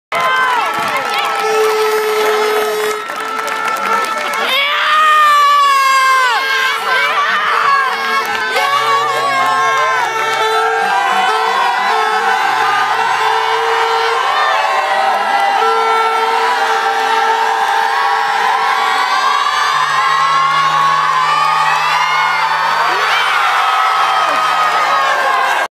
Bikarinn á loft
Bikarinn fer á loft í Kaplakrika 29.september 2012. Gunnleifur Gunnleifsson lyftir bikarnum við mikinn fögnuð.